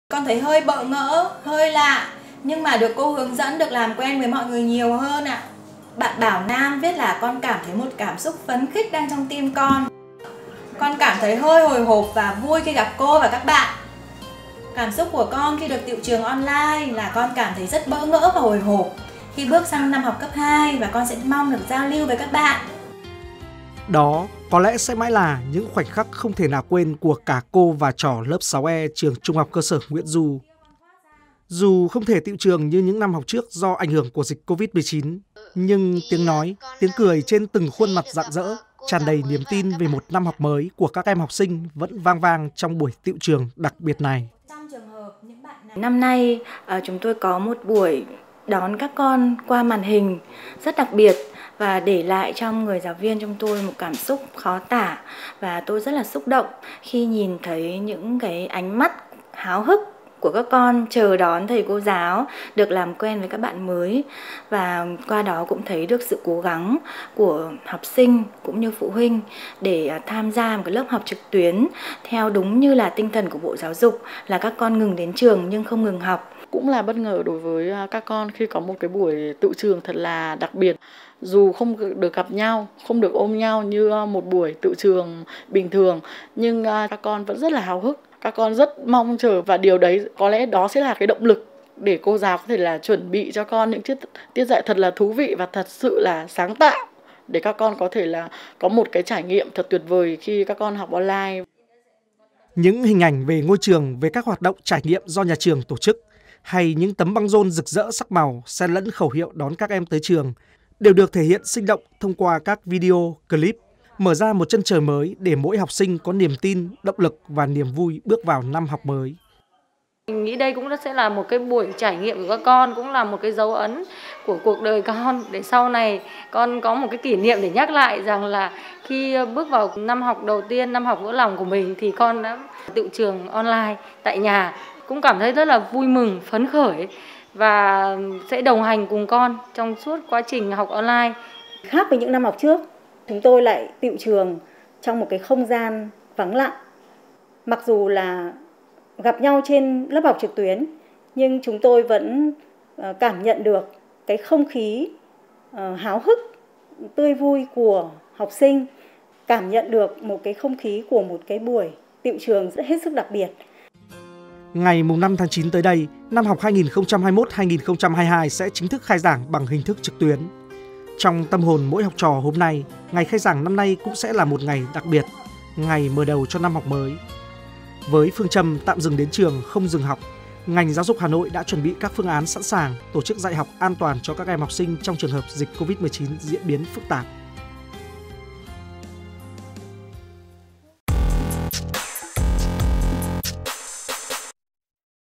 Buổi tựu trường đặc biệt
Diễn ra trong bối cảnh toàn thành phố Hà Nội đang thực hiện các biện pháp giãn cách, phòng chống dịch Covid-19, năm học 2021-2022 trở nên đặc biệt khi học sinh Thủ đô phải tựu trường trực tuyến, khai giảng trực tuyến và học trực tuyến trong buổi học đầu tiên.